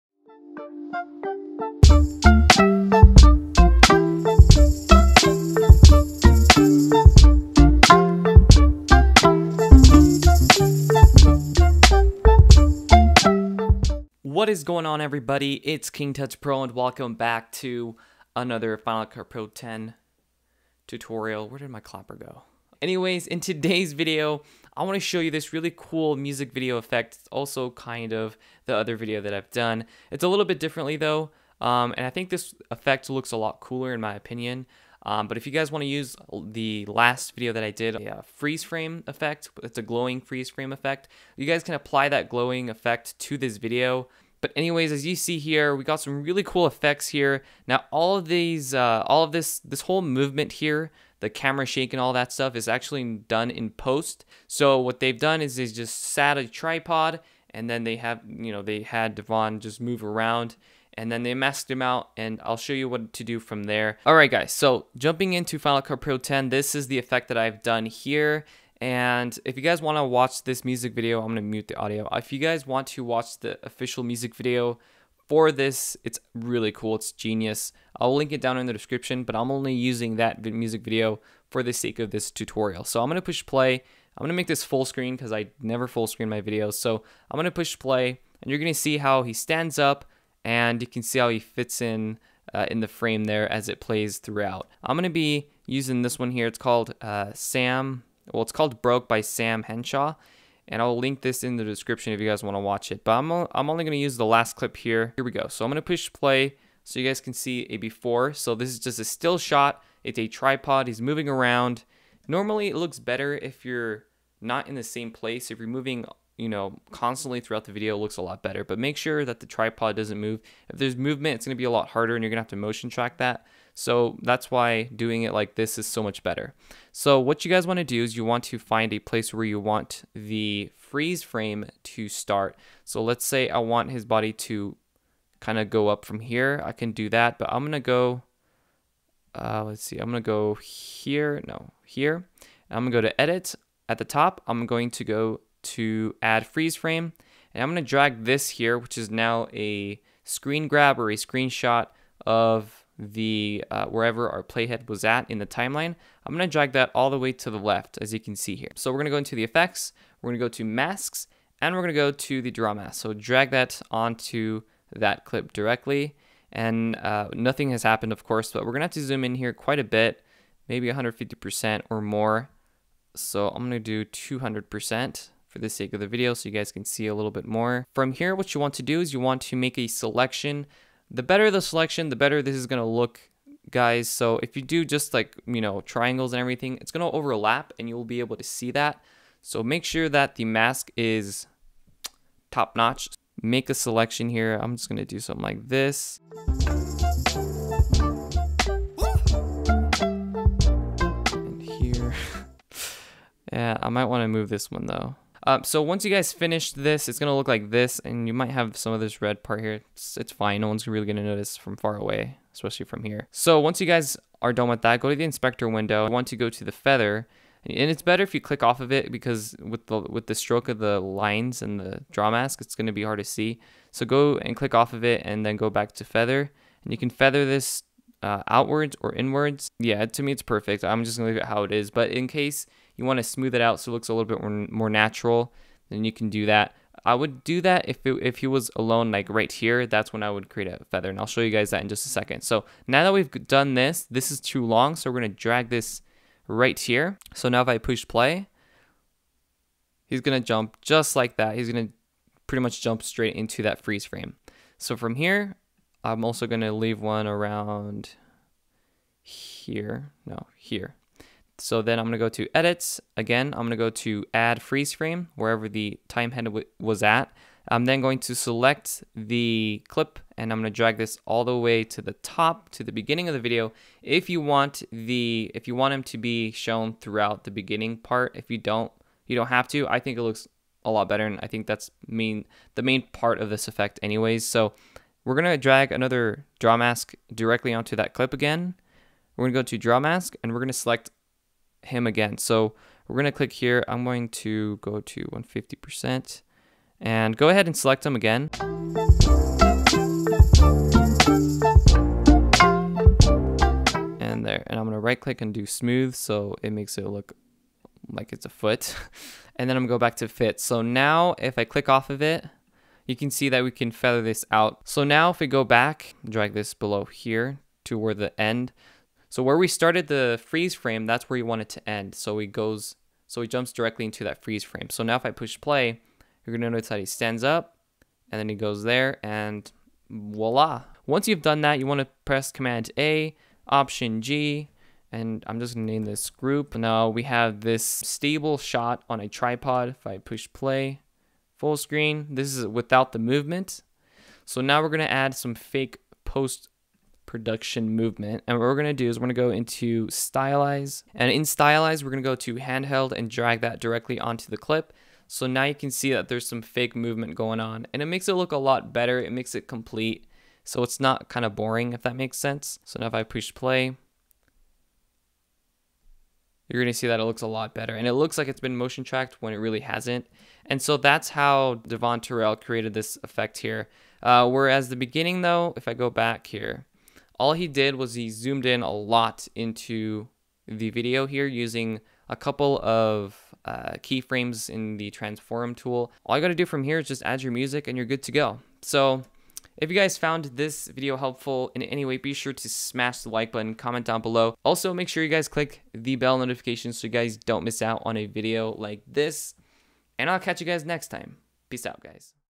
How To Make A Freeze Frame Effect (with a twist) - Final Cut Pro X
In today's FCPX tutorial, I'll show you how to make a really cool handheld like freeze frame movement effect WITHOUT any plugins in Final Cut Pro X!